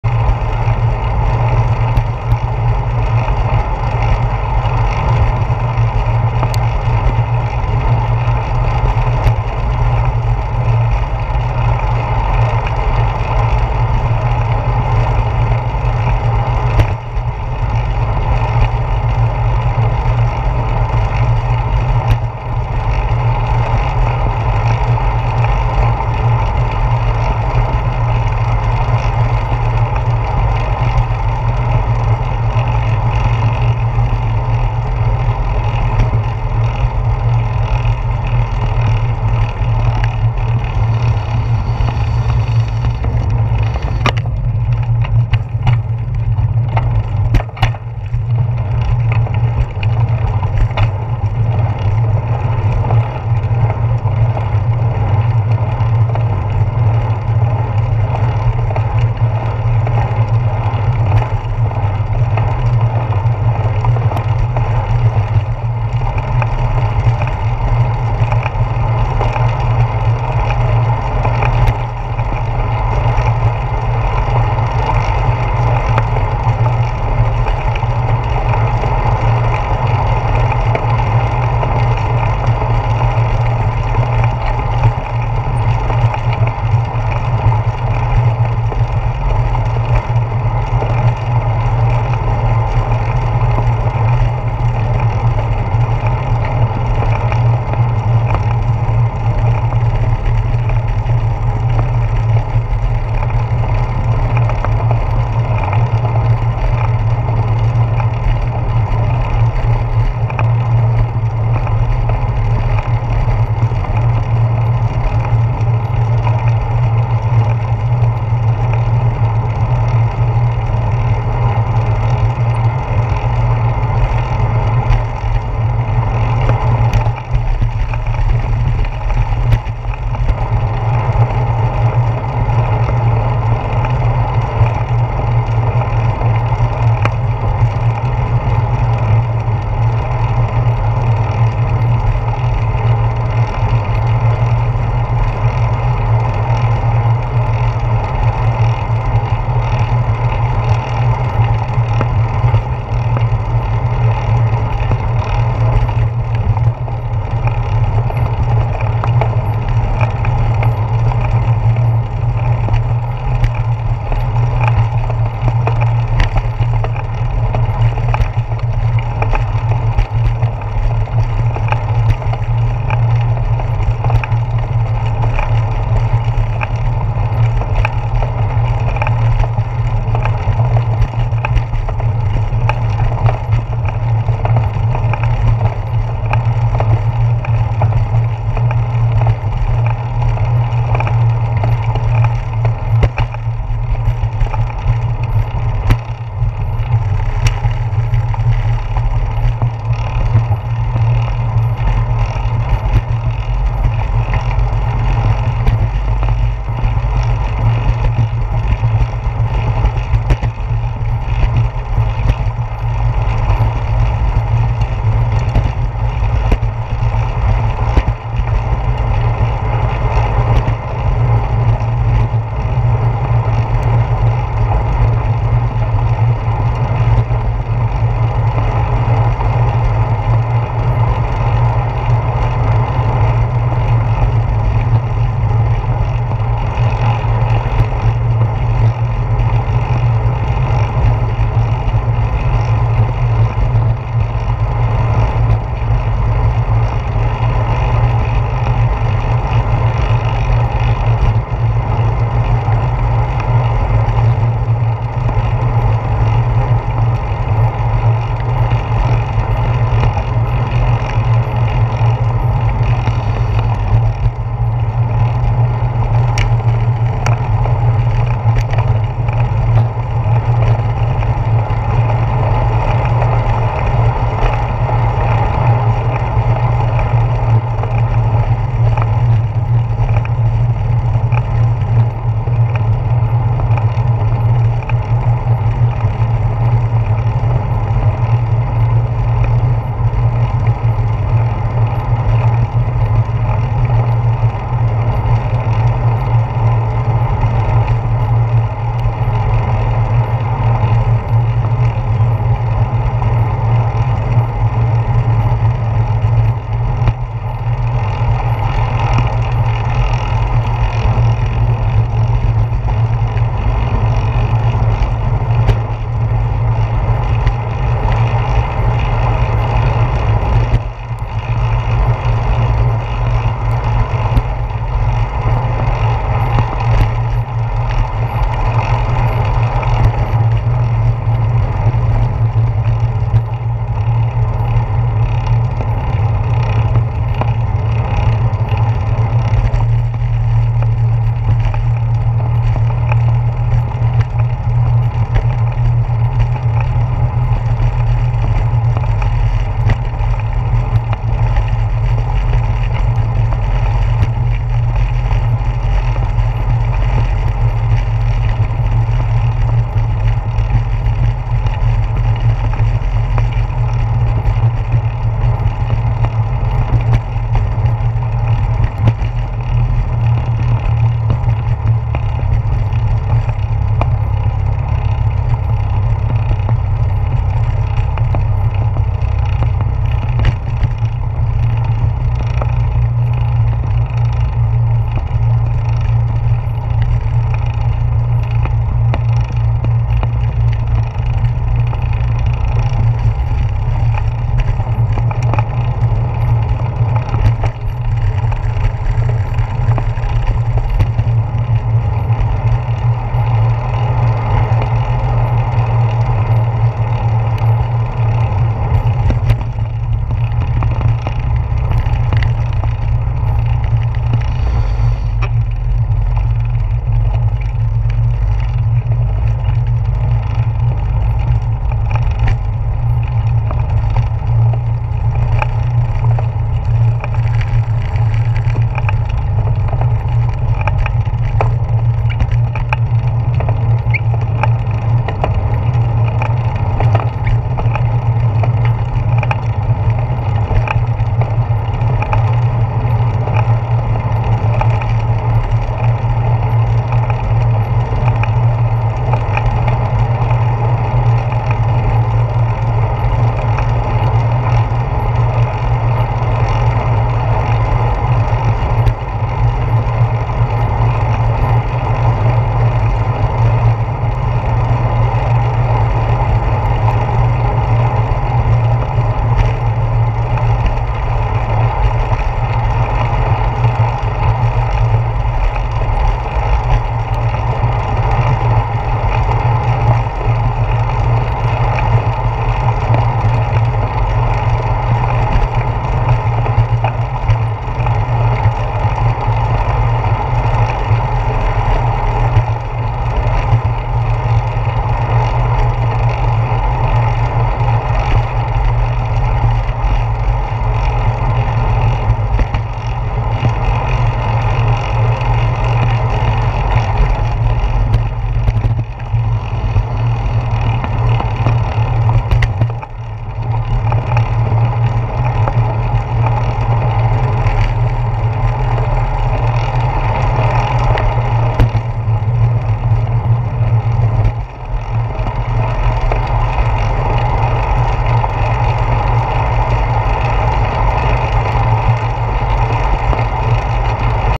Florida Bike commute Monday morning - Thunderstorm race - front camera, part 3/3
A thunderstorm has been circling the area and it was a race to the office this morning. Made for quick biking through the streets of Safety Harbor and Clearwater, Florida on the way to work on the morning of June 22, 2015.